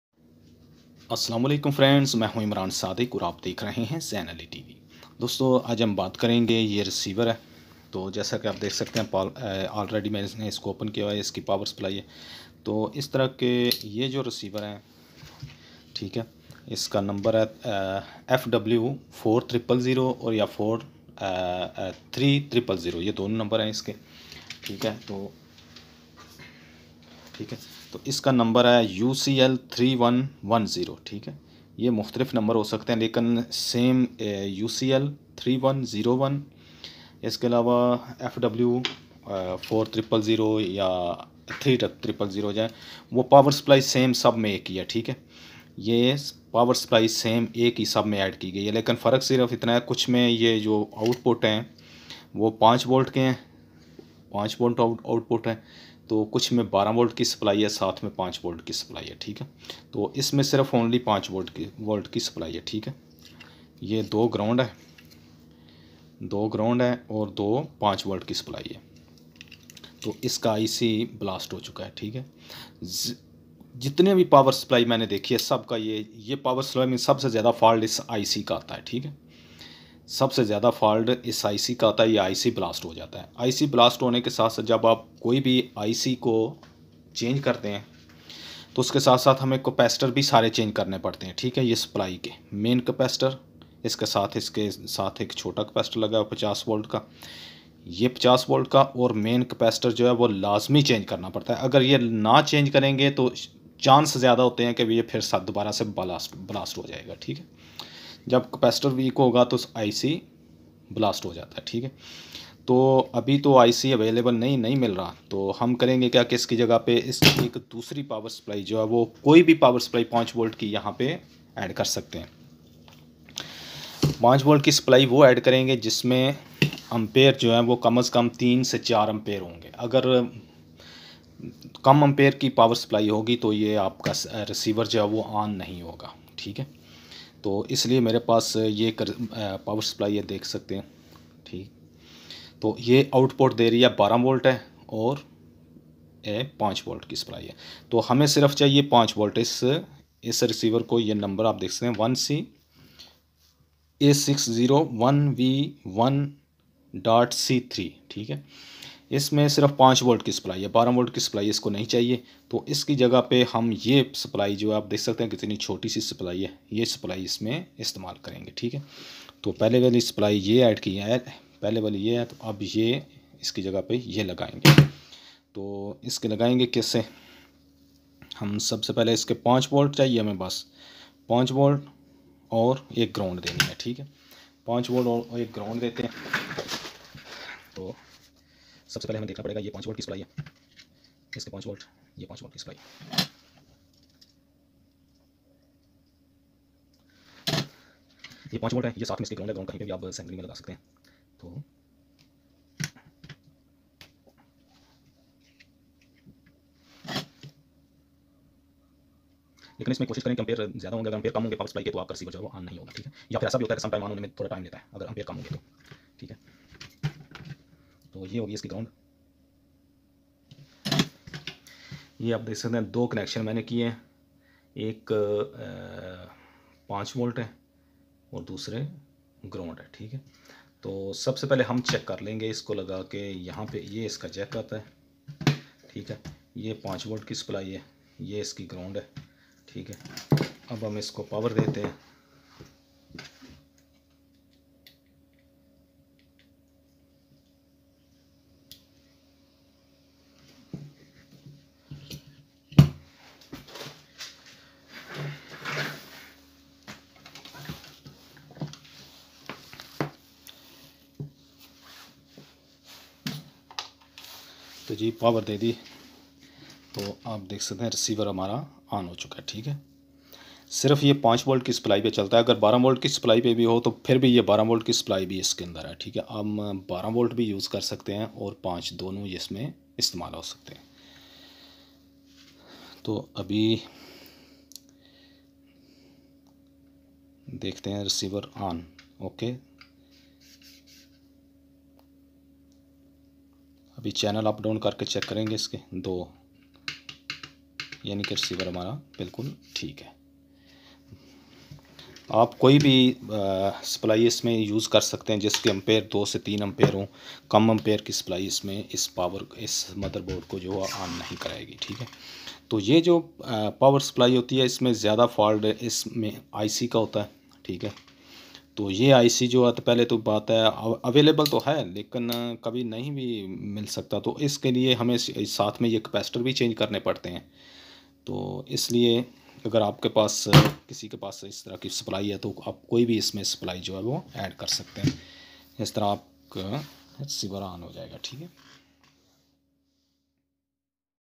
HDMI UCL3101 Receiver Replaced Power supply Urdu/Hindi
Zainalitv HdmiReceiver